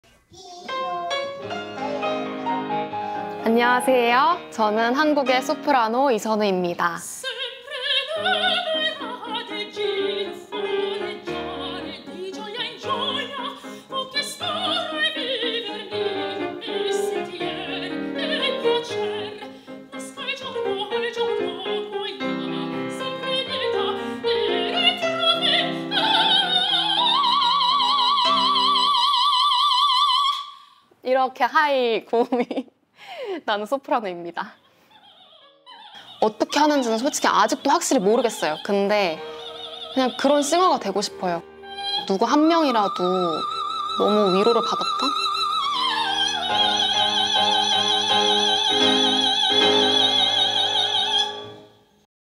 “A soprano sings high notes like *that*!” Introducing Seonwoo Lee | 이선우를 소개합니다
Seonwoo Lee, a Korean soprano, is the third Andrea Bocelli-Community Jameel scholar at the @RCMLondon, supported by Community Jameel and the @FondazioneAndreaBocelli.

This is a sneak peek in Seonwoo's life as she prepares to travel to London to begin her studies at the Royal College of Music in September 2022.

Launched by Community Jameel and the Andrea Bocelli Foundation in 2019, the Bocelli-Jameel Scholarship supports upcoming opera singers from around the world to study at the RCM, one of the world’s leading conservatoires.